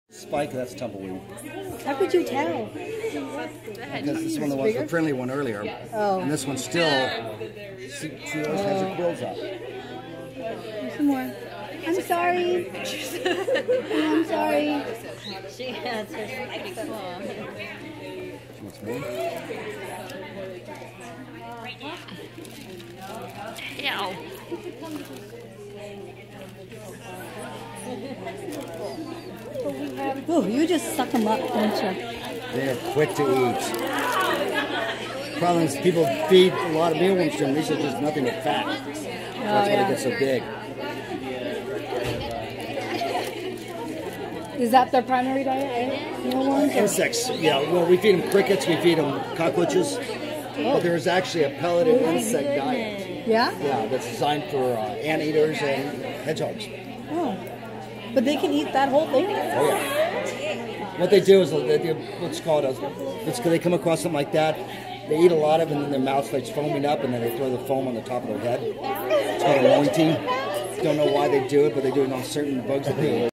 Hedgehog vacuums up mealworms :))
hedgehog from the Palo Alto Junior Zoo & Museum gobbling up mealworms.